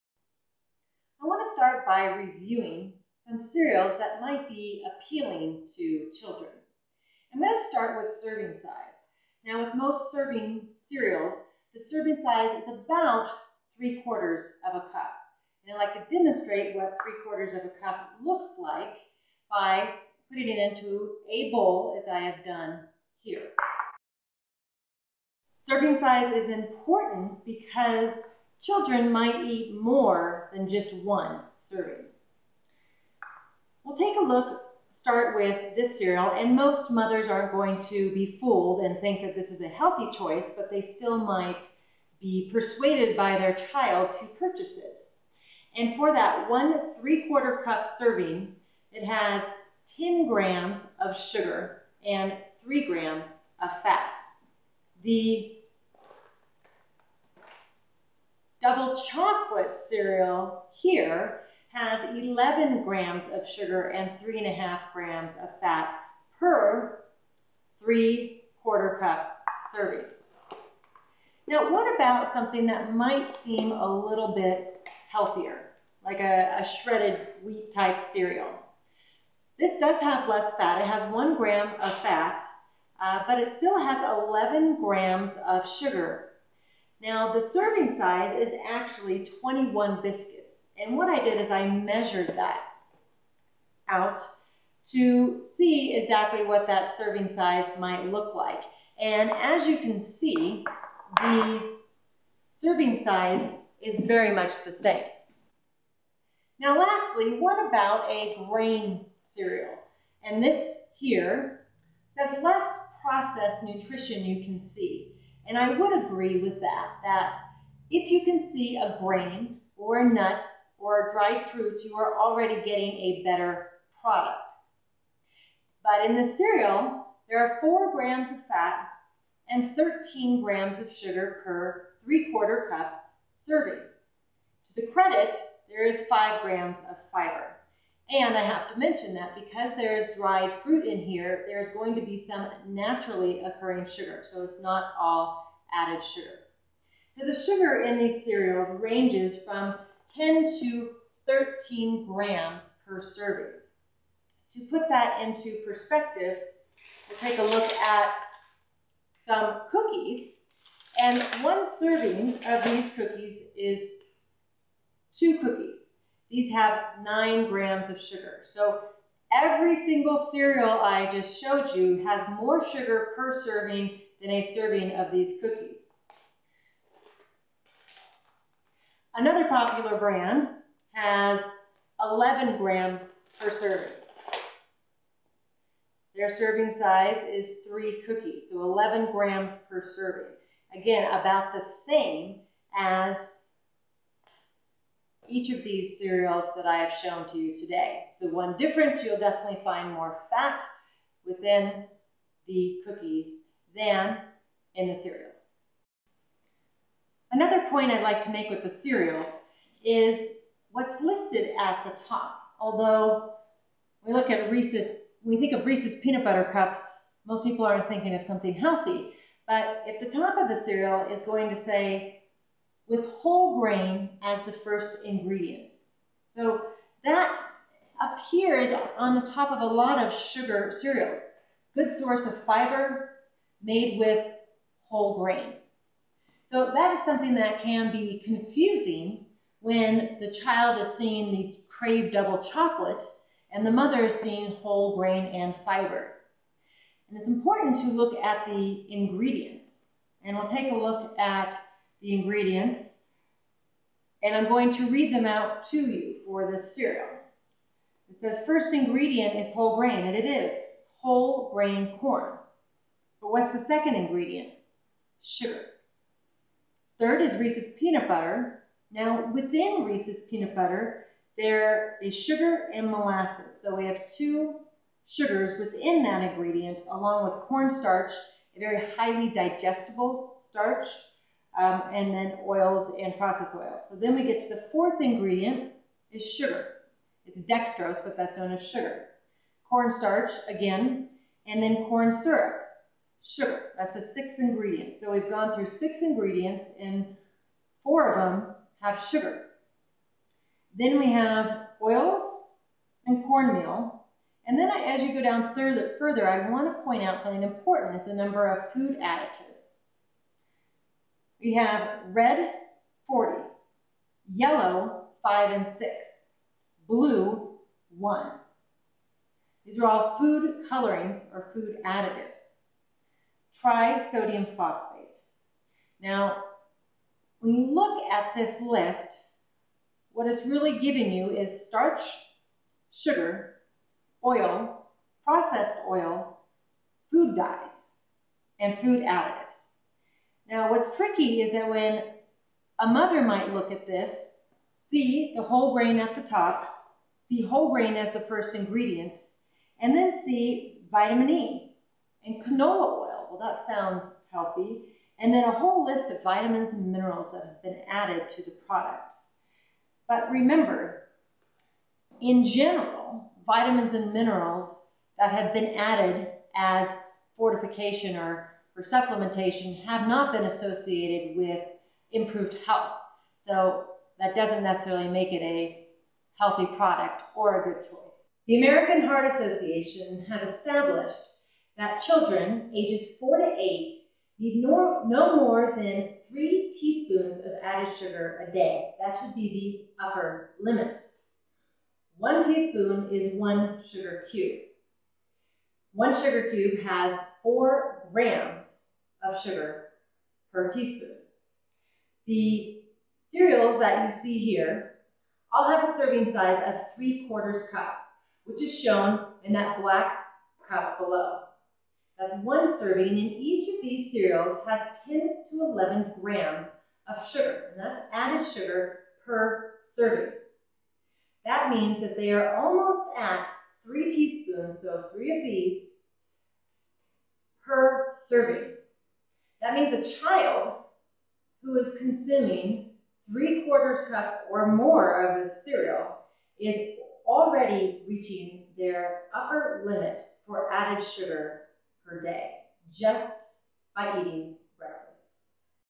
Processed Foods Kids Are Eating Part 1
This video reviews the types of foods, in particular breakfast foods, such as cereals, that children are consuming, along with their ingredient list and basic nutrient content, with a focus on sugar.